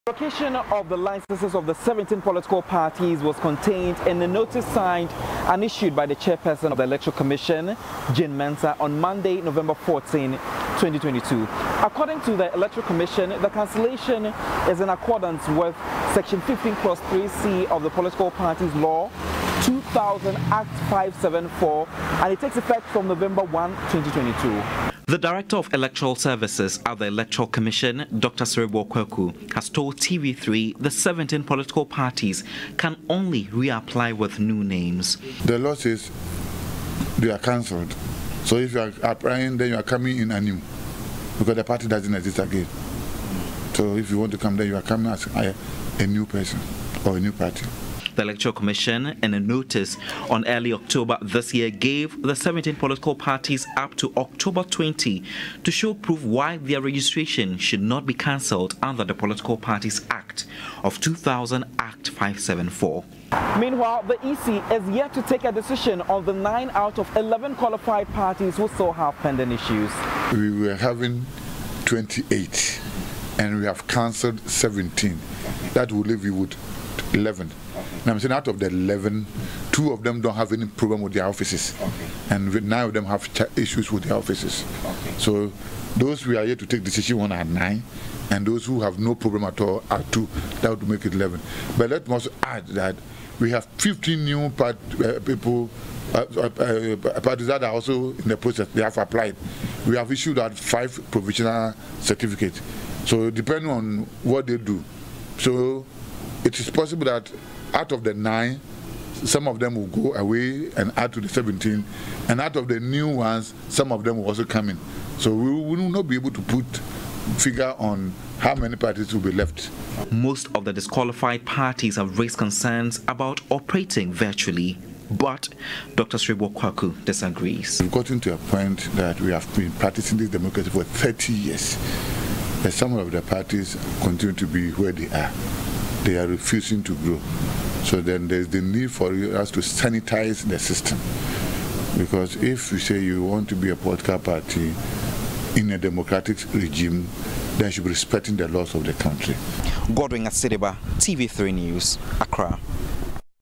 Political party operations: Dr Quaicoe says revocation of licenses are in the line with law.
The Electoral Commission is yet to make a decision on the 9 out of 11 qualified parties with pending issues.